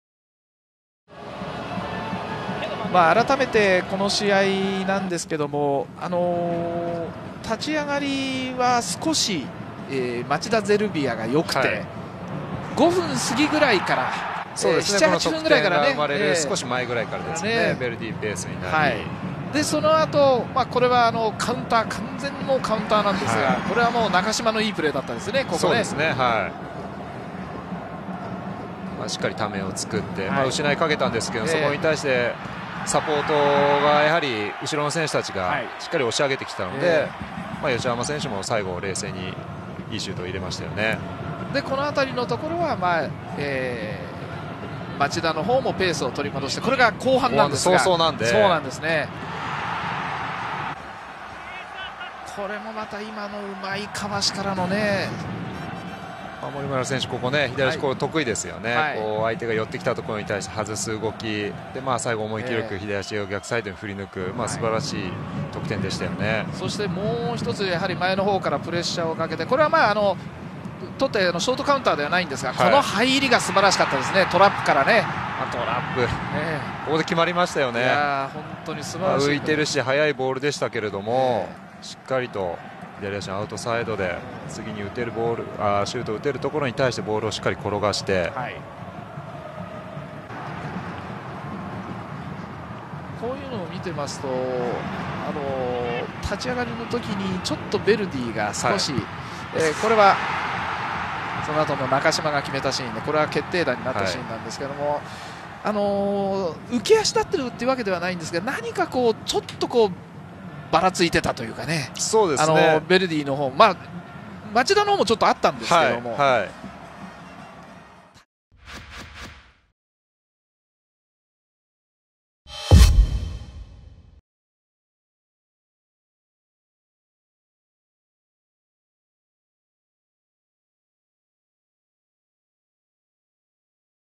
【公式】ハイライト：東京ヴェルディvsＦＣ町田ゼルビア 明治安田生命Ｊ２リーグ 第12節 2018/5/3
2018年5月3日（木）に行われた明治安田生命Ｊ２リーグ 第12節 東京Ｖvs町田のハイライト動画です。

東京ヴェルディ  1-4  ＦＣ町田ゼルビア
東京Ｖ：林　陵平[14']
町田：吉濱　遼平[17'][58'] 森村　昂太[48'] 中島　裕希[88']